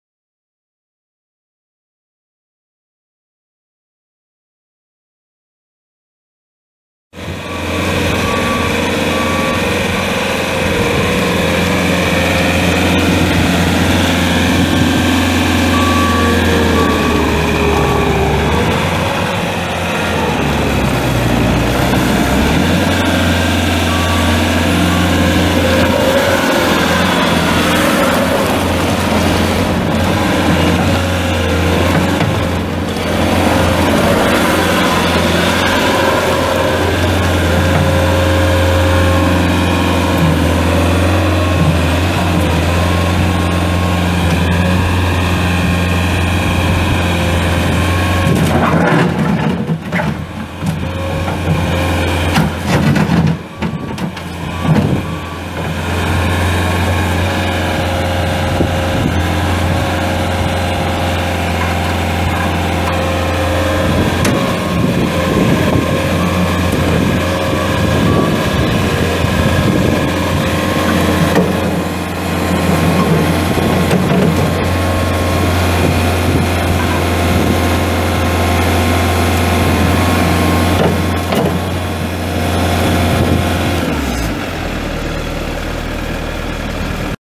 2014 Bobcat T650 skid steer for sale | no-reserve Internet auction April 5, 2018
View a detailed listing of this item and place bids

2014 Bobcat T650 skid steer
1,469 hours on meter
Enclosed cab
Wipers
Front and rear work lights
Backup alarm
72"W bucket
Bolt-on cutting edge
Auxiliary hydraulics
Hydraulic quick coupler
Bridgestone 17.5"W tracks
Serial ALJG11147
It's located in Stuart, IA, and we're selling it to the highest bidder regardless of price.